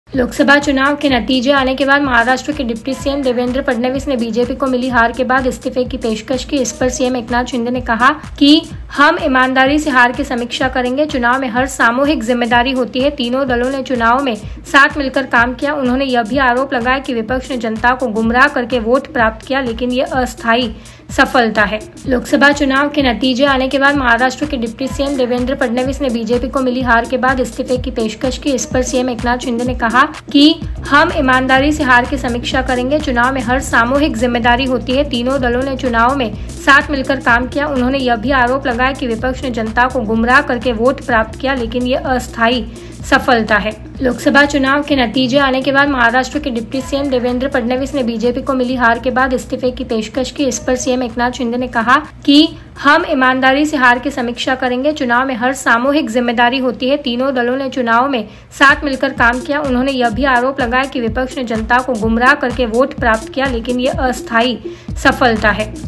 Devendra Fadnavis के इस्तीफे की पेशकश पर सीएम एकनाथ शिंदे बोले हम ईमानदारी से हार की समीक्षा करेंगे
लोकसभा चुनाव के नतीजे आने के बाद महाराष्ट्र के डिप्टी सीएम देवेंद्र फडणवीस ने बीजेपी को मिली हार के बाद इस्तीफे की पेशकश की है. इस पर सीएम एकनाथ शिंदे ने कहा कि है कि हम ईमानदारी से हार की समीक्षा करेंगे. चुनाव में हार सामूहिक जिम्मेदारी होती है. तीनों दलों ने चुनावों में साथ मिलकर काम किया है. उन्होंने ये भी आरोप लगाया कि विपक्ष ने जनता को गुमराह करके वोट प्राप्त किया लेकिन ये अस्थाई सफलता है.

महाराष्ट्र के मुख्यमंत्री एकनाथ शिंदे ने आगे कहा, ''वोट शेयर पर नजर डालें तो मुंबई में महायुति को दो लाख से ज्यादा वोट मिले. विपक्ष के इस नरेटिव ने कि संविधान बदल दिया जाएगा, हमें नुकसान पहुंचाया है.''